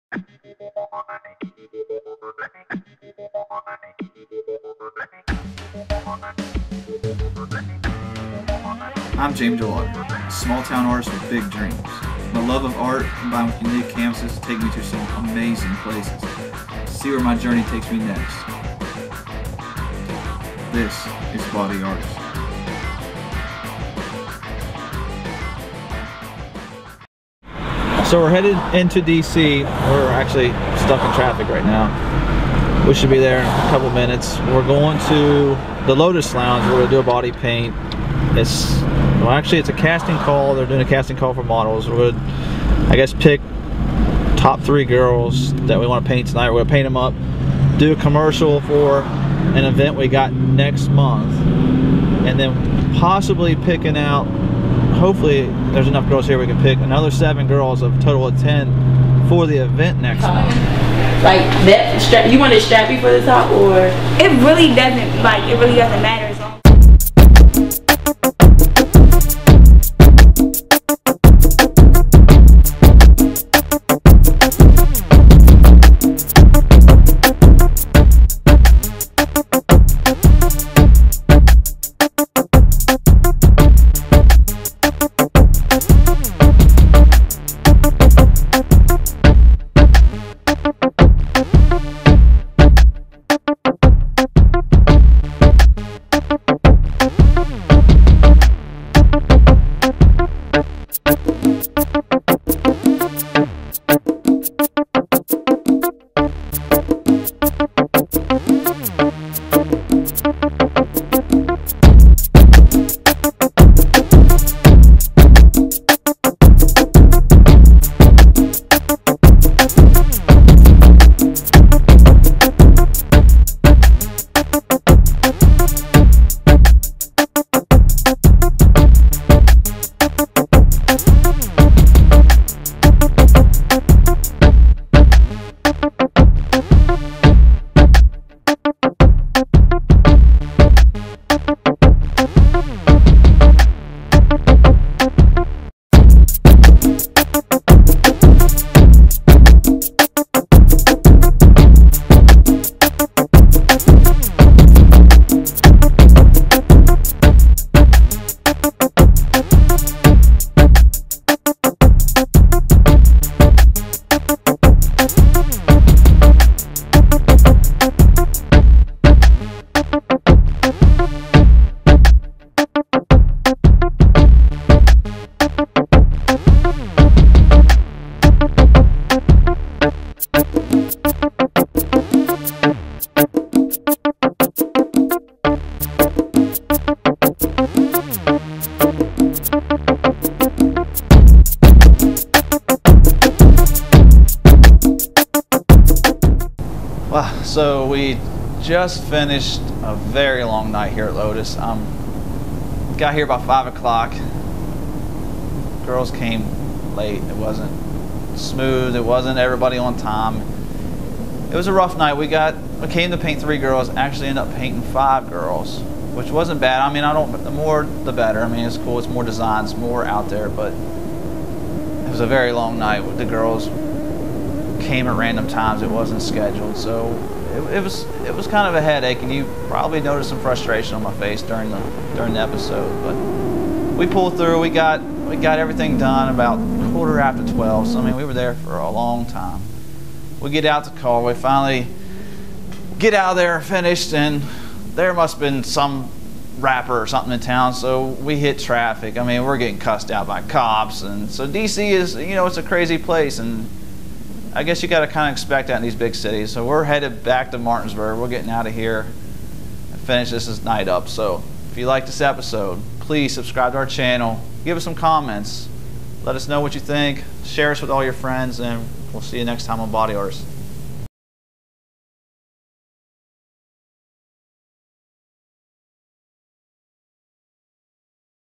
Body Artist - Body Painting at Lotus Lounge, Washington DC
Body Painter Jamie Delawder of Drawin' Attention visits the Washington DC Club Lotus Lounge, where he paints 5 girls for a commercial promoting an up coming event.